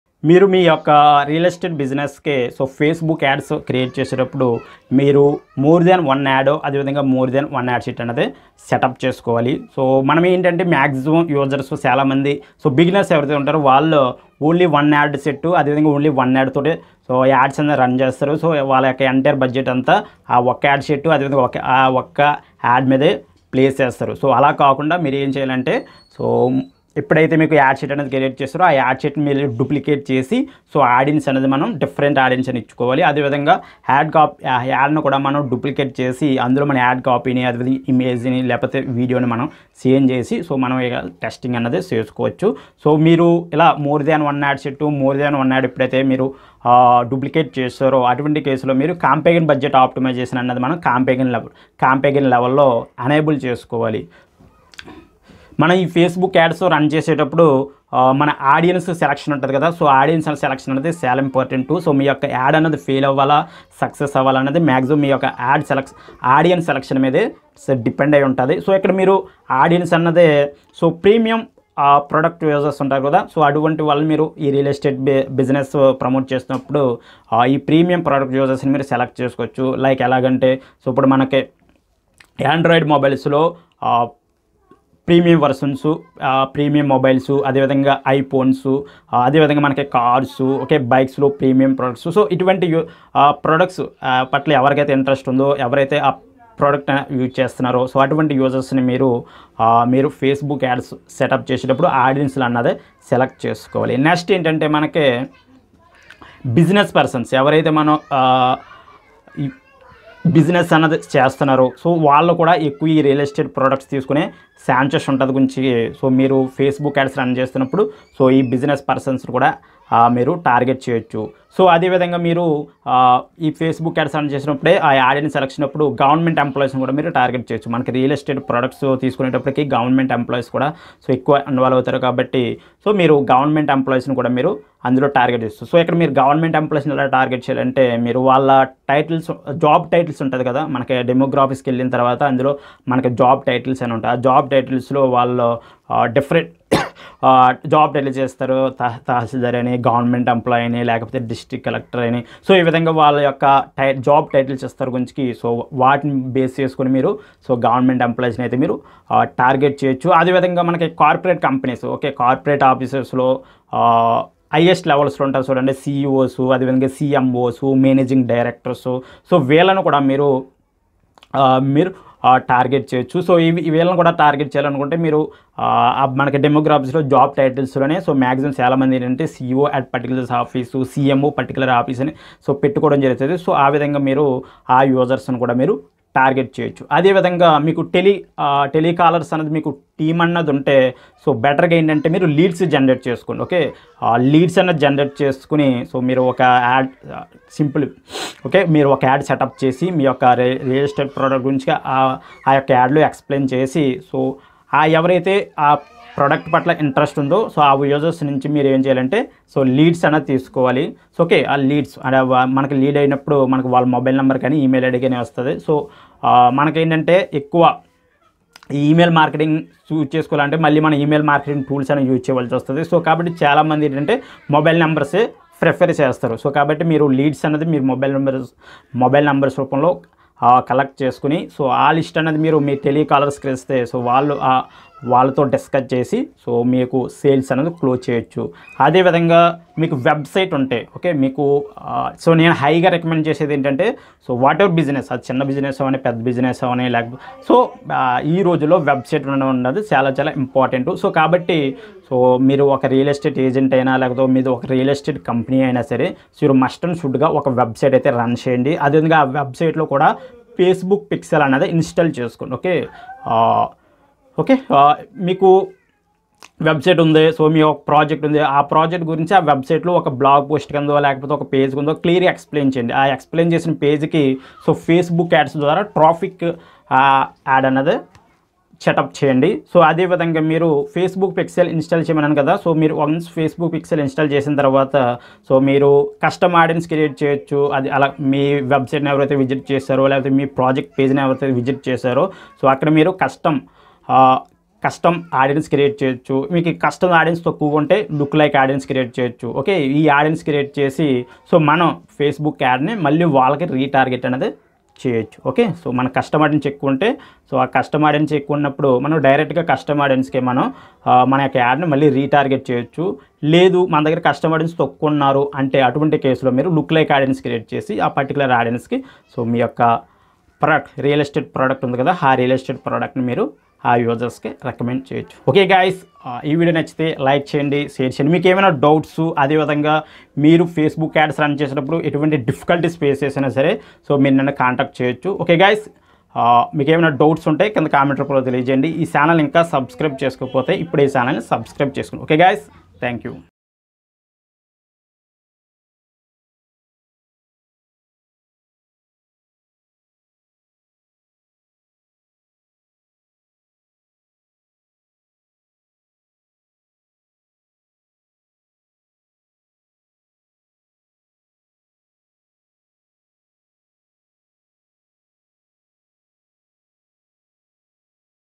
Facebook Ads Strategies for Real Estate Business Telugu
Do you want to promote your business with Facebook ads? then here are the some best strategies. This video explained in the Telugu.

2. This video and description contain promotion of my paid products and services.